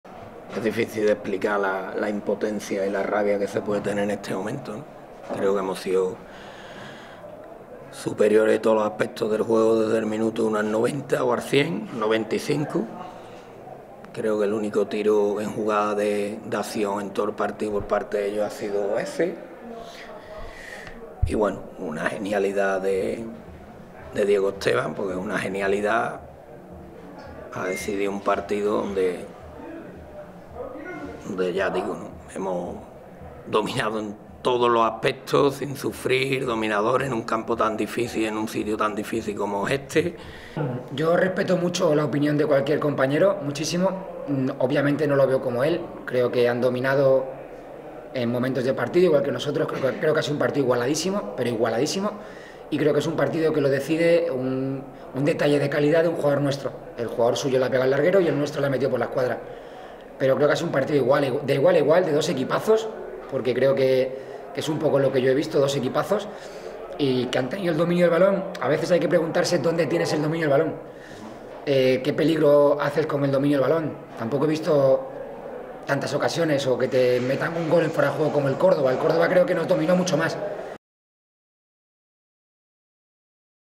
Lolo Escobar discrepa de Romero en cuanto al merecimiento de unos y otros
Mañana escucharemos a Diego Esteban, pero vamos a la rueda de prensa posterior al Derby del Estrecho donde Jose Juan Romero afirmó que el fútbol le debe algo por lo sucedido en el Nuevo Mirador, ya que para el técnico del Ceuta los suyos dominaron todos los aspectos del partido y pusieron las oportunidades. Pero no pusieron los goles según un Lolo Escobar con visión distinta a su colega que aseguró que su equipo dominó todos los registros.